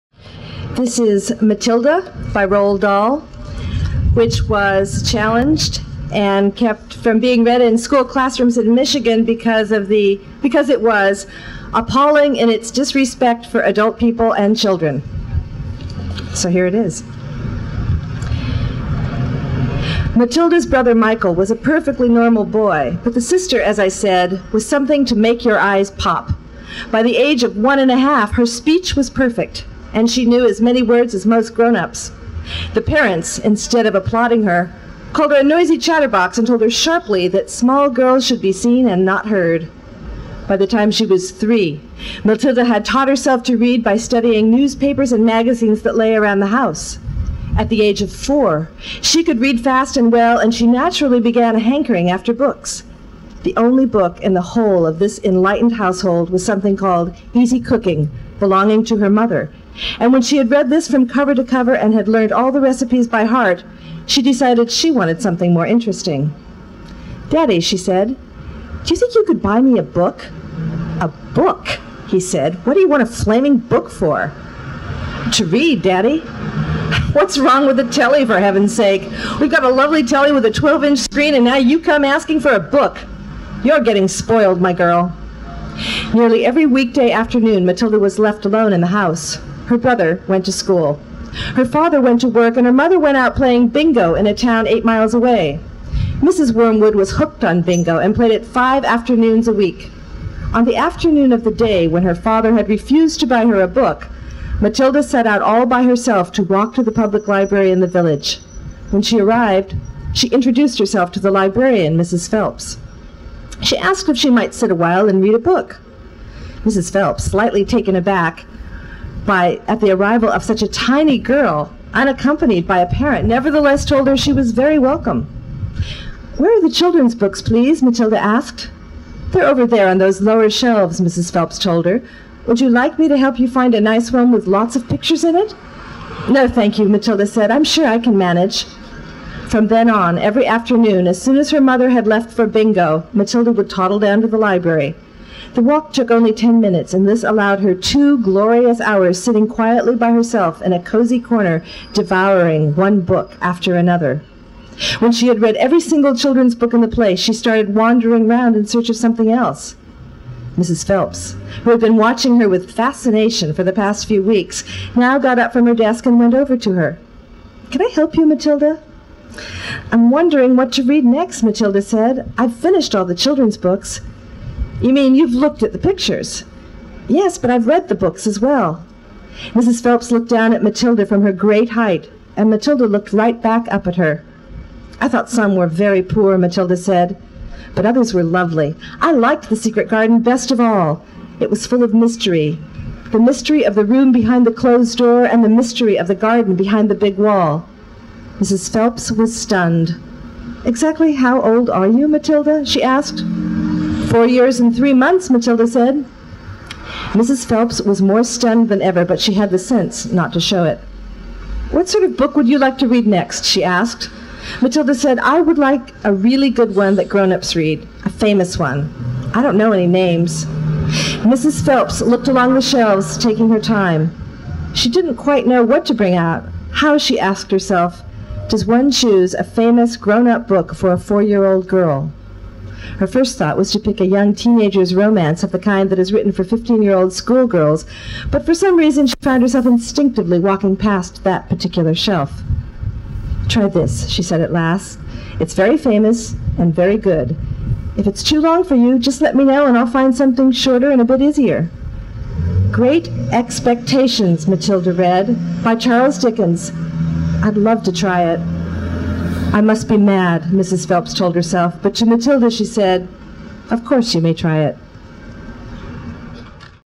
1997 Susan Harloe reads Roald Dahl for Banned Books Week at the San Francisco Public Library
September 22, 1997. SFPL librarian Susan Harloe reads "Matilda" by Roald Dahl for Banned Books Week 1997. Check out books by Roald Dahl in our online catalog